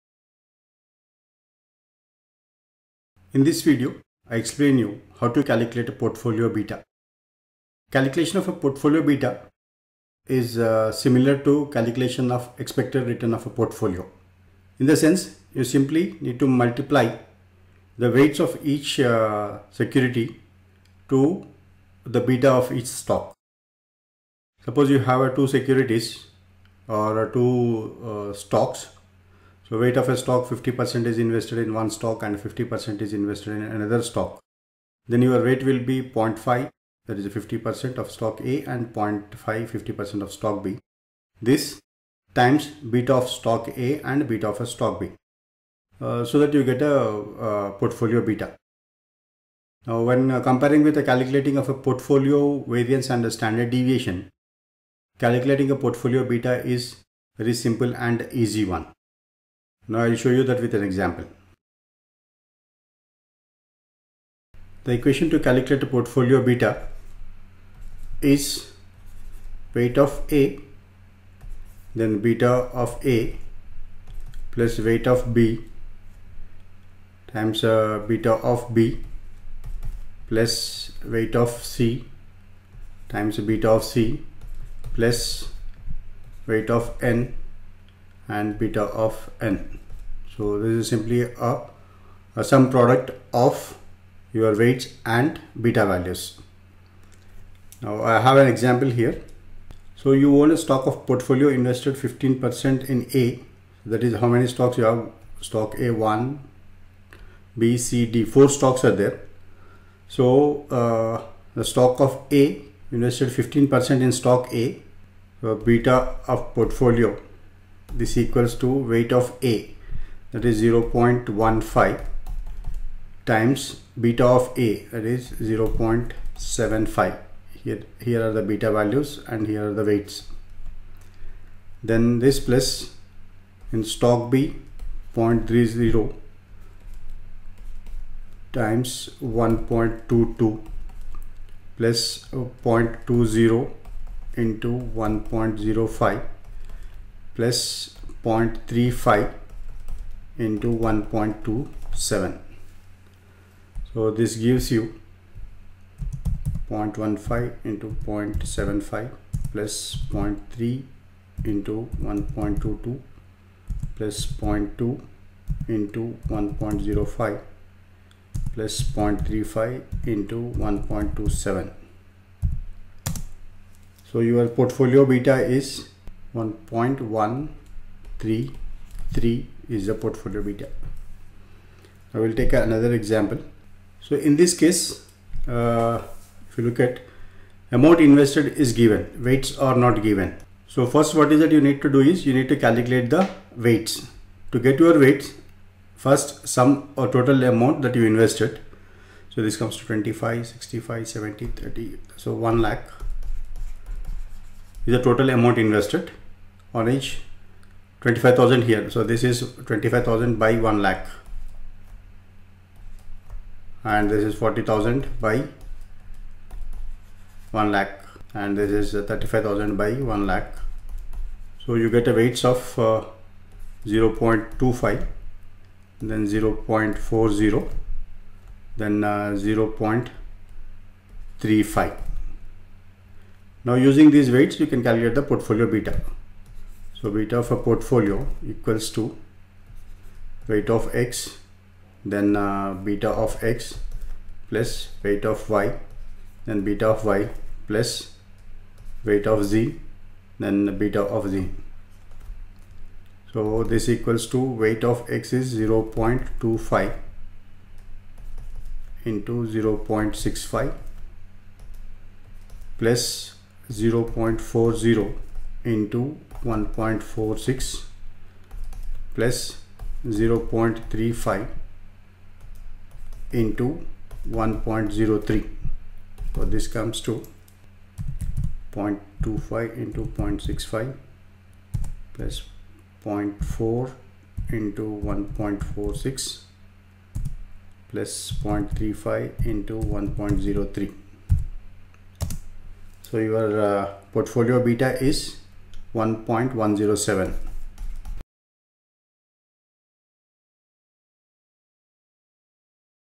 Portfolio Beta
This video show you how to calculate portfolio beta. @RKVarsity